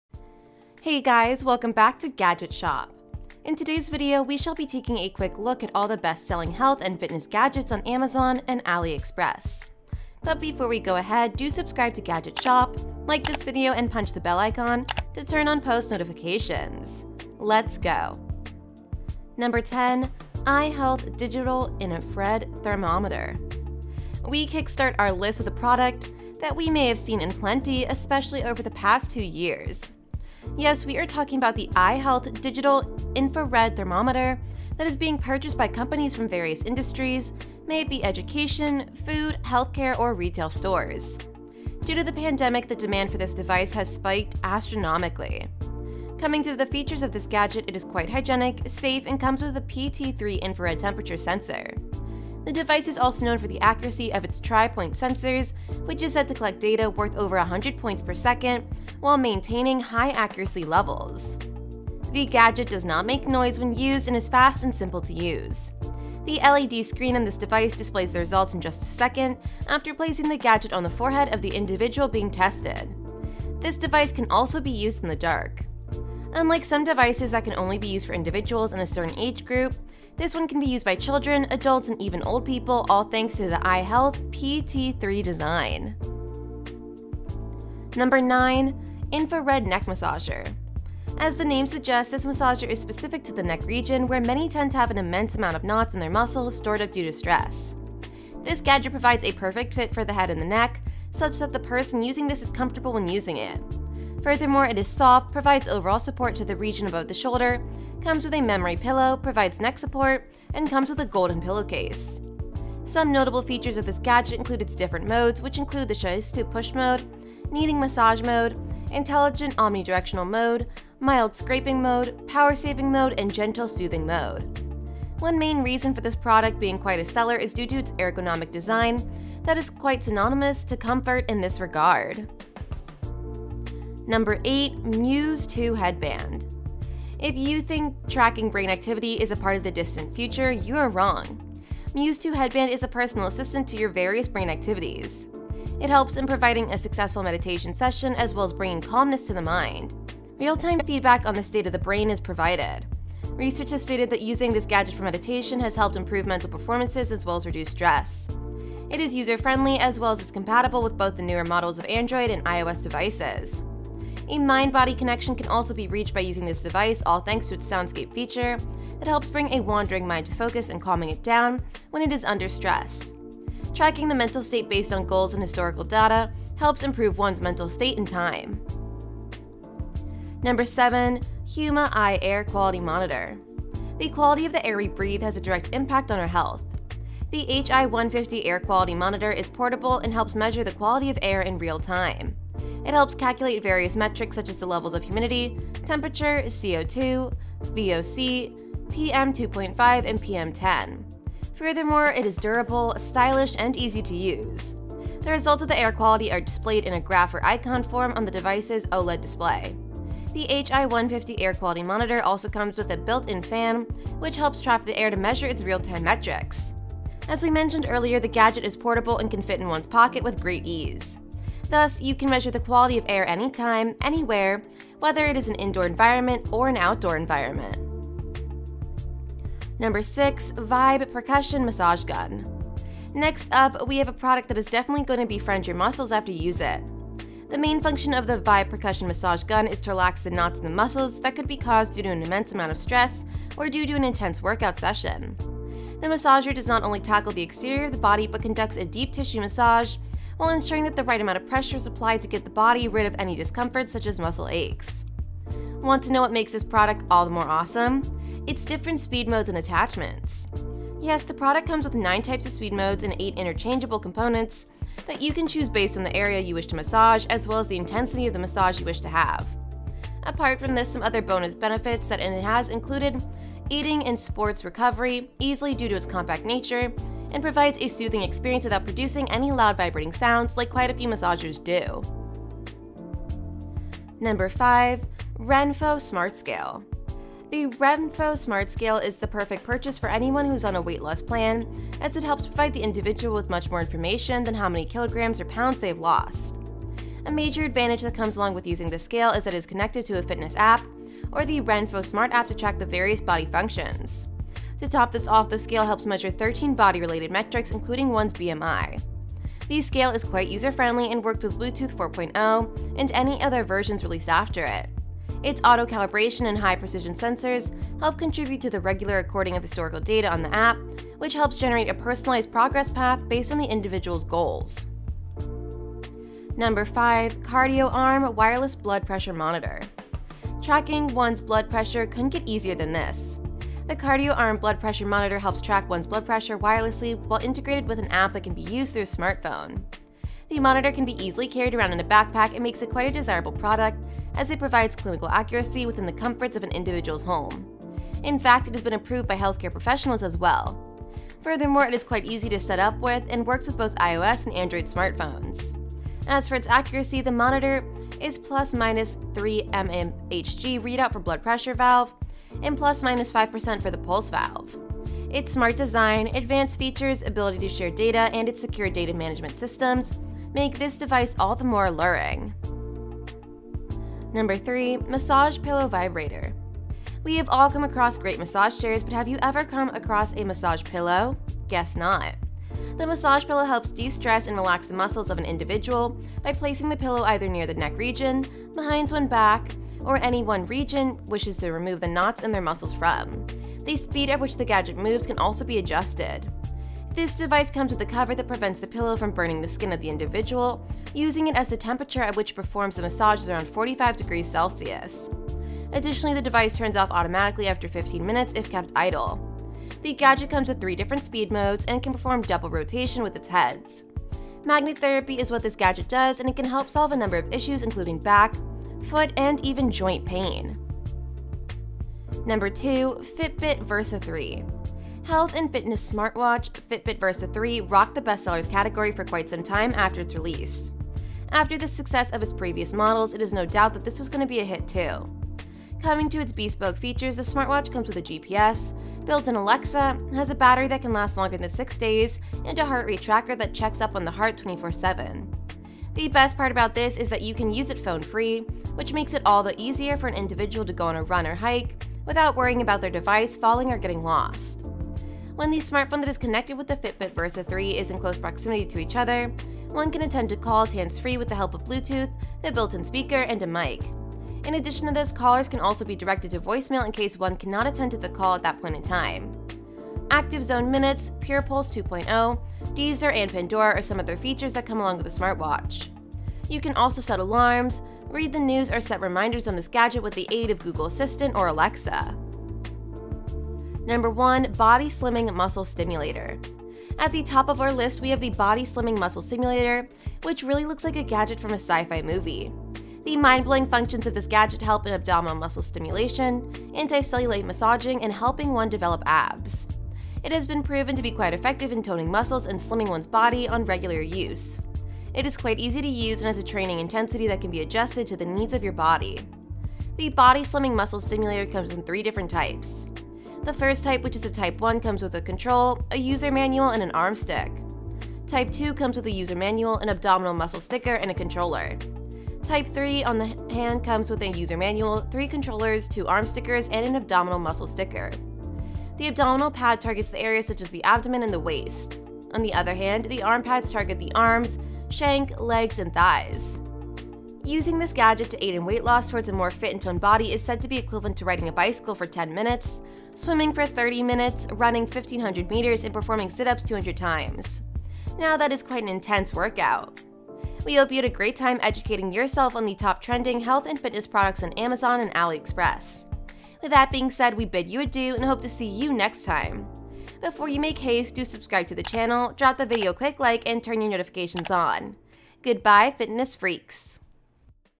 10 Best Selling Health Gadgets on Amazon and AliExpress
10 Best Selling Health Gadgets on Amazon and AliExpress

In this video we will show you 10 Best Selling Health Gadgets on Amazon and AliExpress.

In today’s video, we shall be taking a quick look at all the best-selling health and fitness gadgets on Amazon and AliExpress.

10] iHealth Digital Infrared Thermometer

9] Infrared Neck Massager

8] Muse 2 Headband

7] Huma-i Air Quality Monitor

6] VYBE Percussion Massage Gun

5] RENPHO Smart Scale

4] QardioArm Wireless Blood Pressure Monitor

2] Fitbit Versa 3